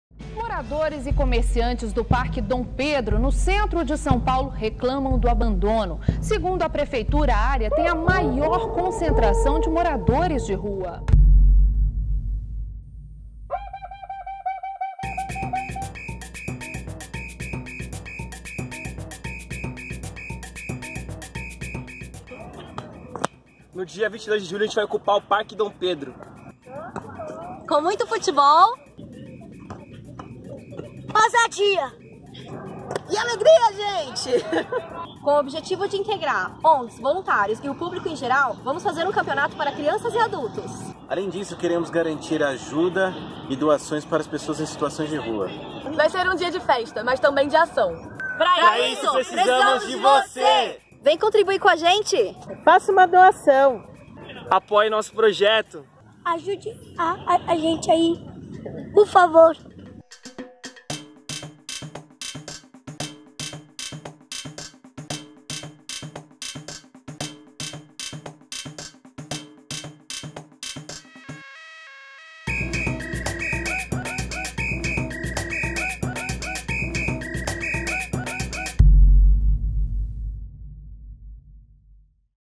Copa das Causas 2018 - ATADOS
No dia 22 de julho, domingo de futebol em ritmo de Copa do Mundo, a “Copa das Causas” ocupará o Parque Dom Pedro II, no centro de São Paulo, com o objetivo de promover a diversidade e integração entre os seus participantes: amantes do futebol e das causas sociais.

Roteiro, filmagem e edição: Arthur Mazai, Bernardo Carvalho, Daniele Santos, Elys Martins, Letícia Costa e Tiago Souza